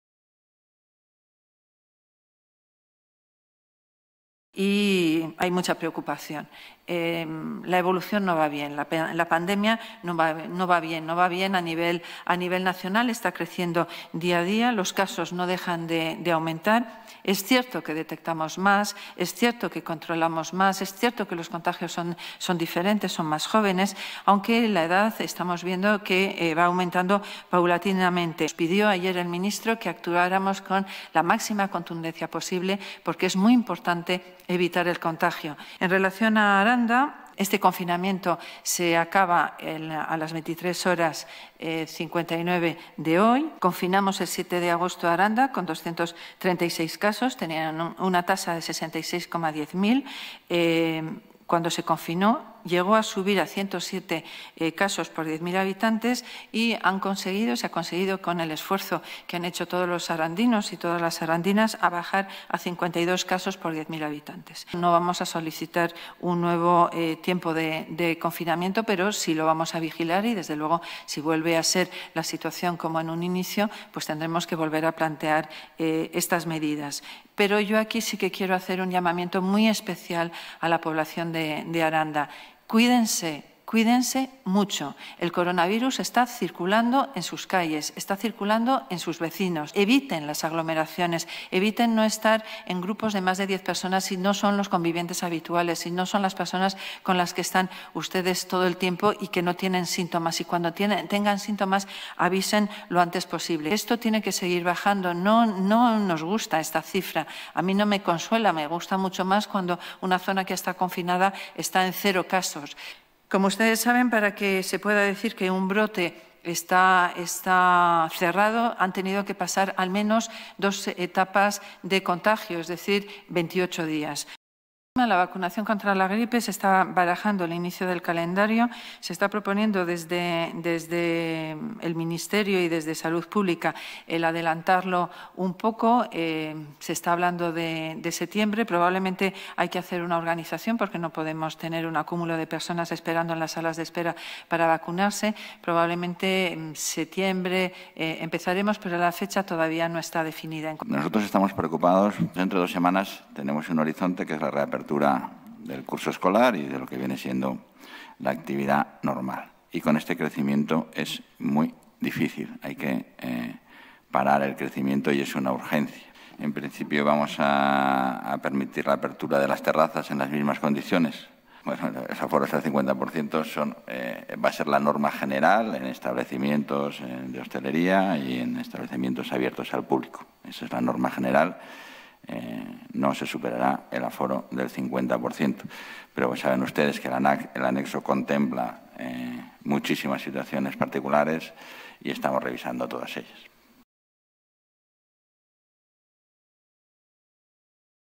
Preocupación en CyL ante los nuevos brotes de COVID-19 - BurgosOnlineTV
El vicepresidente de la Junta, Francisco Igea, y la consejera de Sanidad de Castilla y León, Verónica Casado, han expresado su preocupación ante el aumento de los casos en toda la Comunidad.
«Si no bajamos la incidencia, vamos a tener un problema», ha advertido Francisco Igea, el vicepresidente de la Junta